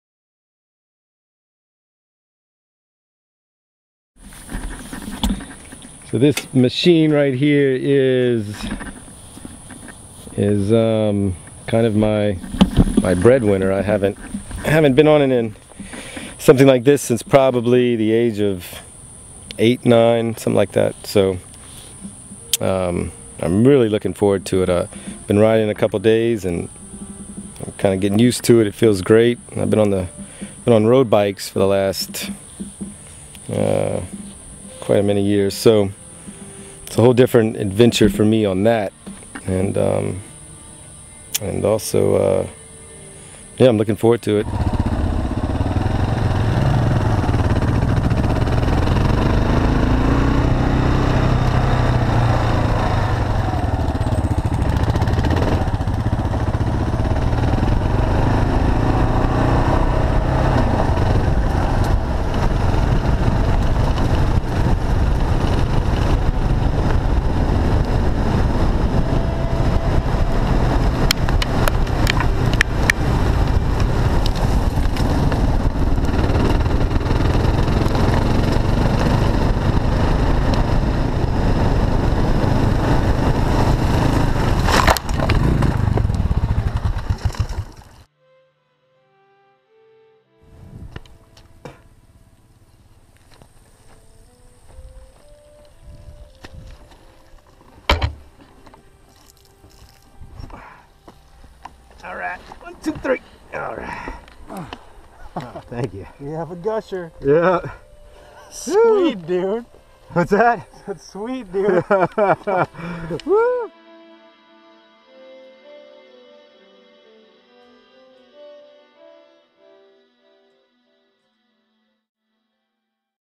Motorcycle crash on the levee in Henderson, Louisiana
Finley on his KLR 650 for some off road riding.
Produced by Studio Arcadia
Producers/ Director: John Finley
Production Advisor: KOH
Appearance: John Finley, Winter